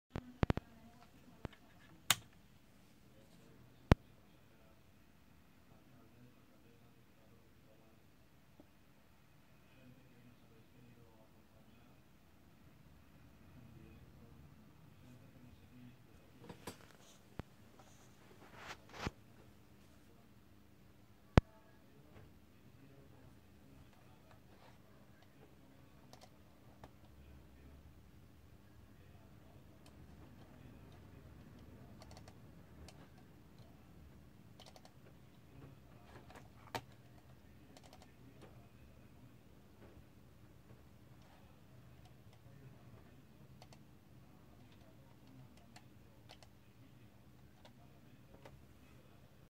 Testing the dual ide interface on the tf328.
Works and detect second ide device.